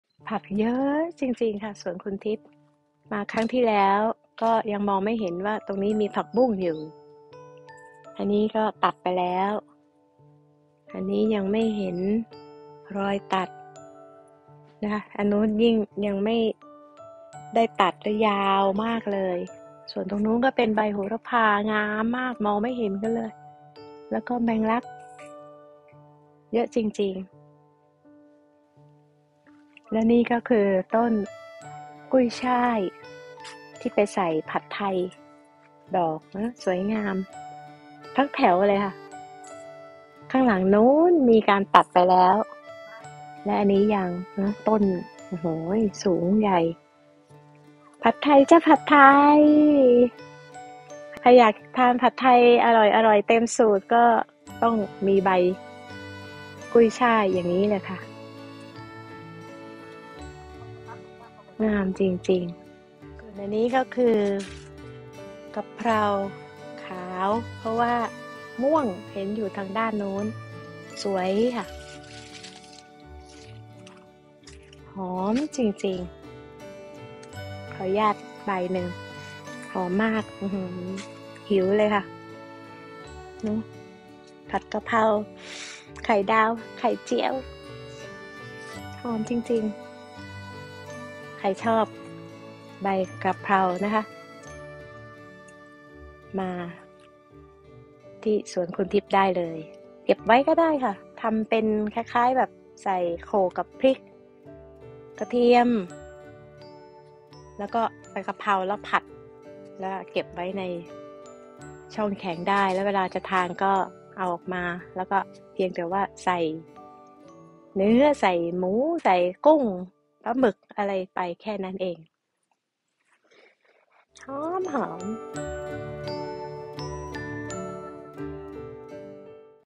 29-08-2022 ไปหลายครั้งแล้ว แต่ก็ยังเห็นไม่ทั่วเลย - ทิพย์สวนผักไทย เบลเยียม
สองโรงเรือนตรงกันข้ามกัน ที่เมื่อก่อนเป็นสตรอเบอร์รี่ แต่ตอนนี้แทนด้วยผักบุ้ง กะเพรา กุ้ยช่าย แถวย้าวยาว สดสวย เขียวขจี ทุกอย่างปลูกในกระถางหมด ข้อดีคือตัดเก็บง่าย ผักสะอาด

ที่อยู่สวน Hoge Beemdstraat 12, 2360 Oud-Turnhout, Belgium